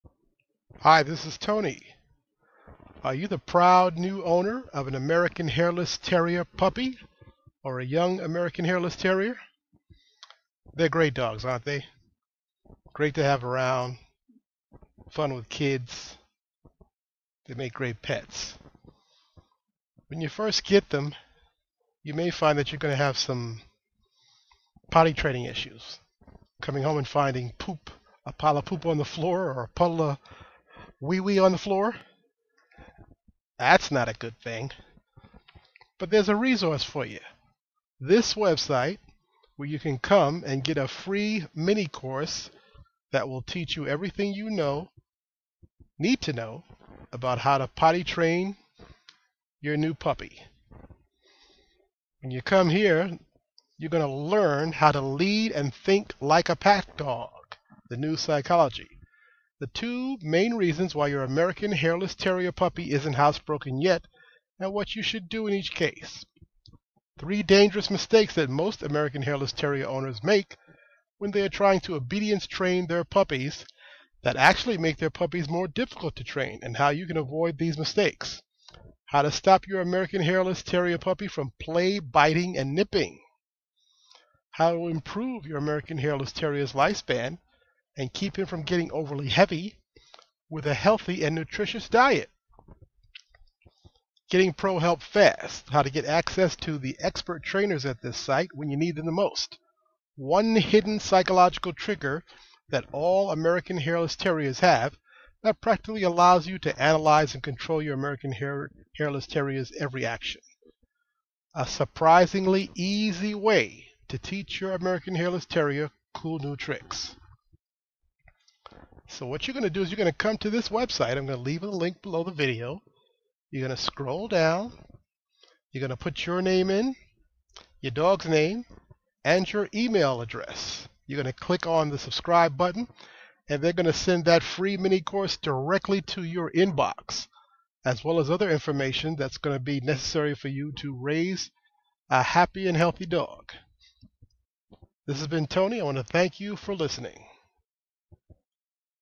***American Hairless Terrier Puppy Potty-Training Free-Mini Course***
American Hairless Terrier potty trained enough? All
  American Hairless Terrier puppies have to go to the bathroom
each time they wake up from a nap, after they
drink, after they eat, or even play hard. If you
take your puppy to the same spot consistently
each time, you will begin to create a habit that
will lead him to successful potty-training.”

Here are a few more resources for your consideration;